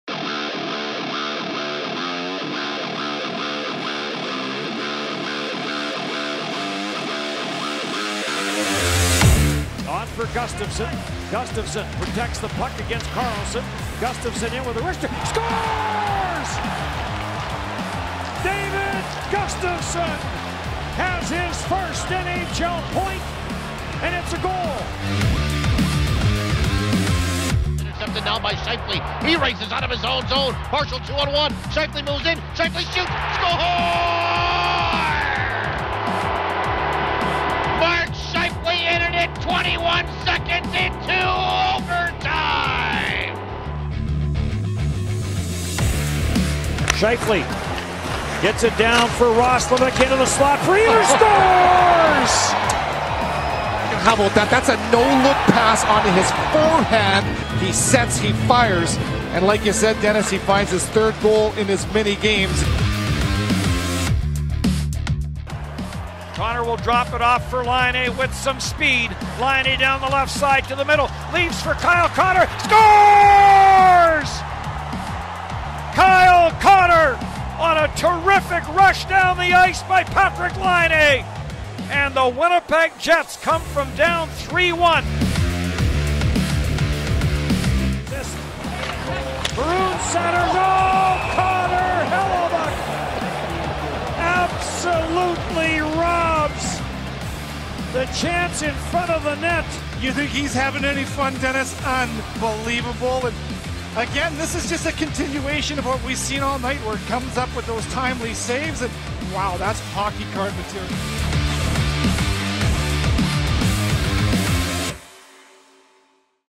HIGHLIGHTS OF THE MONTH: November 2019
November was a month to remember!  Relive Jets successful month with the top plays in Highlights of the Month presented by EA Sports NHL 20!